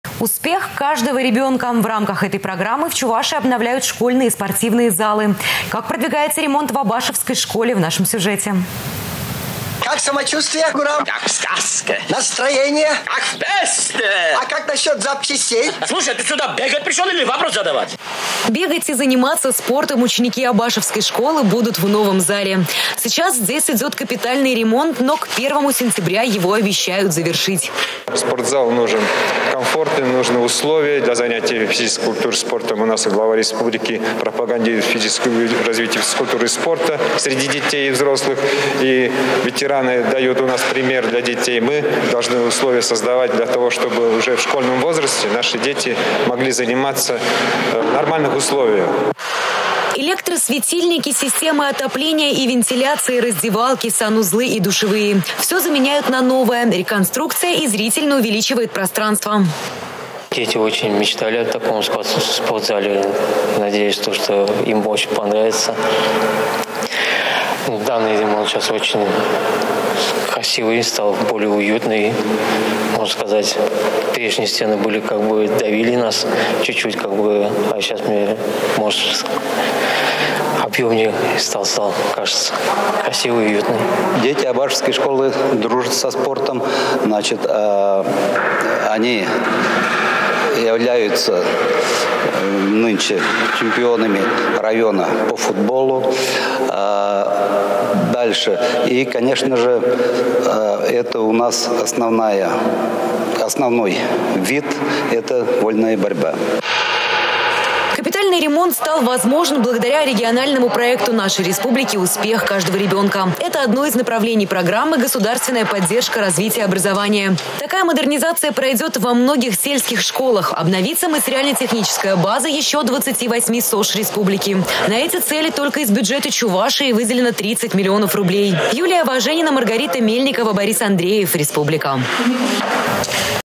Как продвигается ремонт в Абашевской школе
«Успех каждого ребенка». В рамках этой программы в Чувашии обновляют школьные спортивные залы. Как продвигается ремонт в  Абашевской школе  - в нашем сюжете.